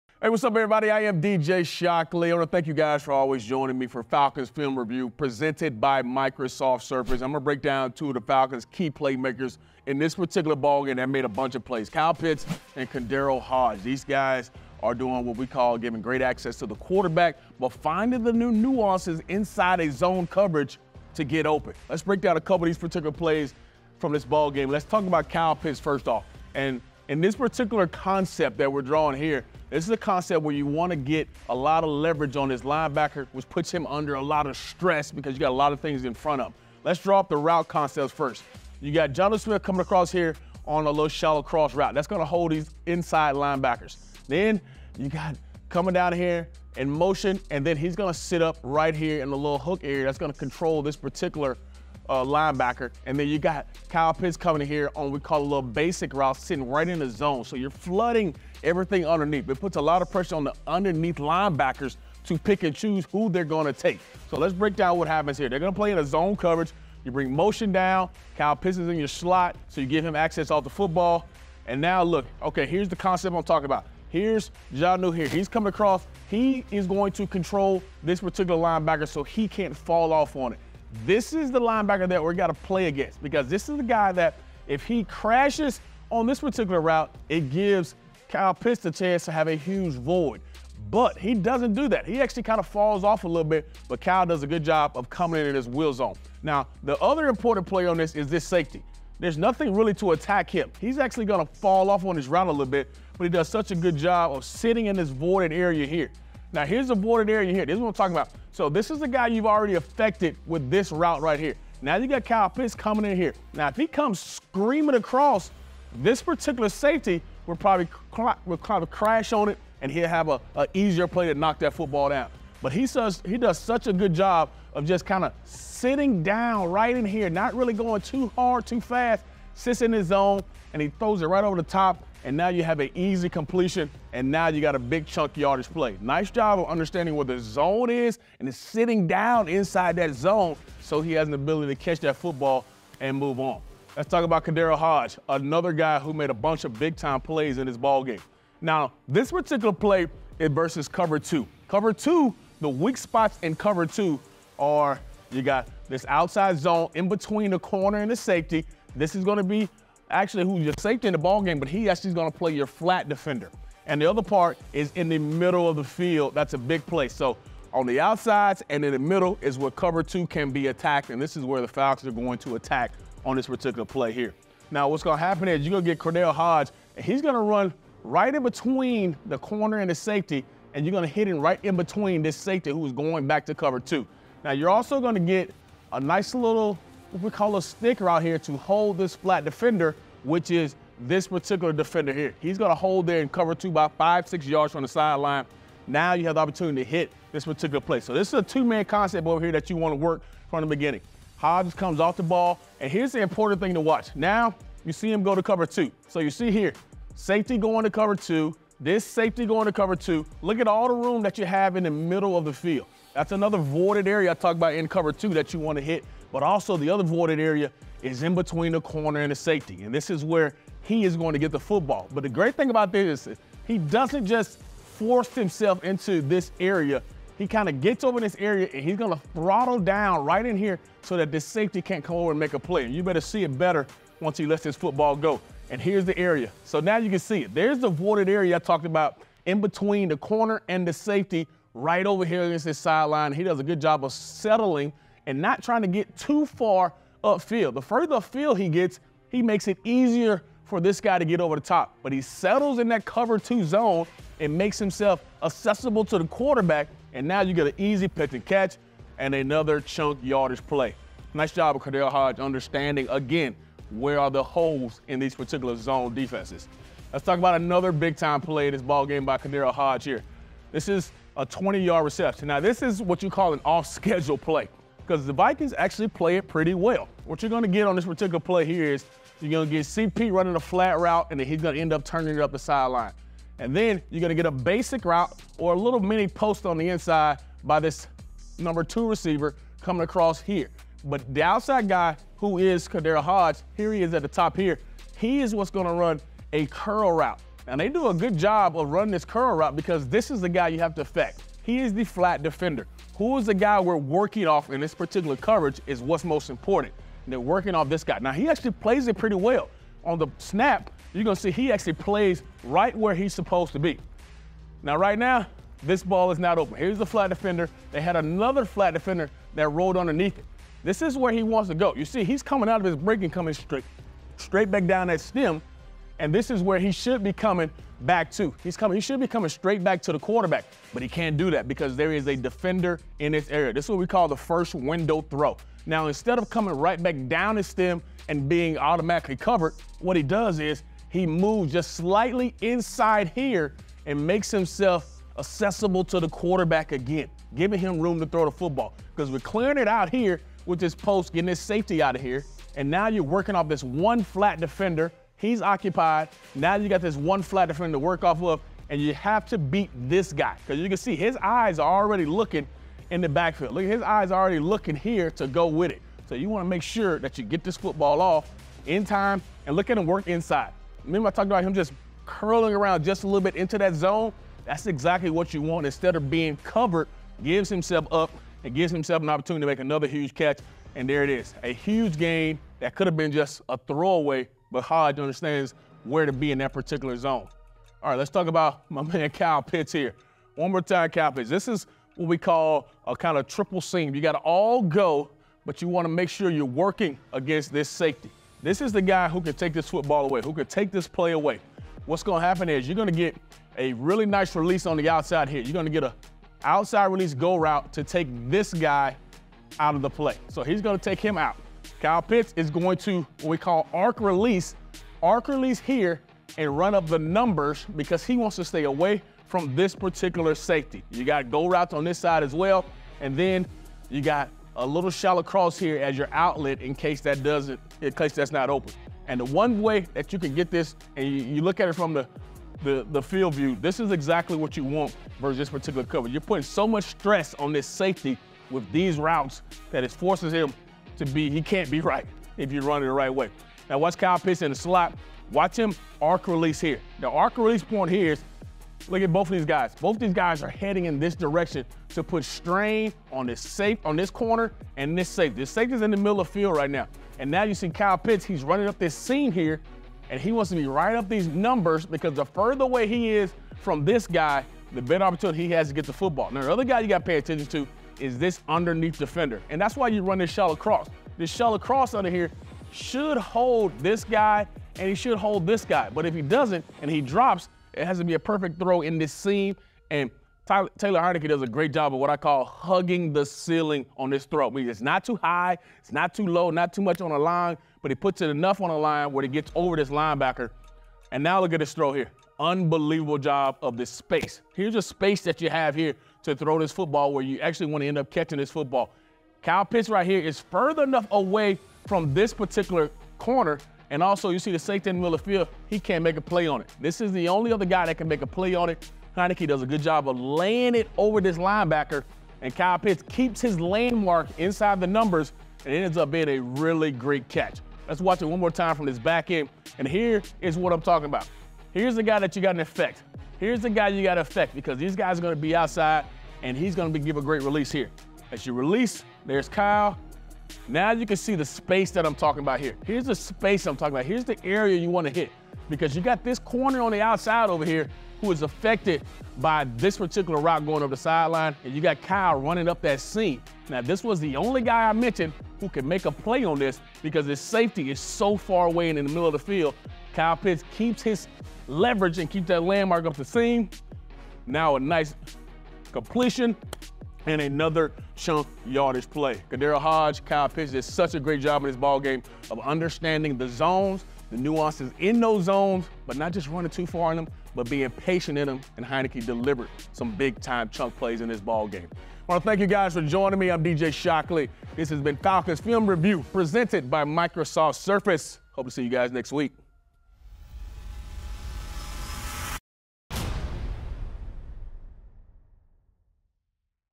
Falcons receivers find gaps in zone coverage | Film Review
D.J. Shockley breaks down the X's and O's of how Kyle Pitts and KhaDarel Hodge have been exploiting big gaps in opponents' zone defenses.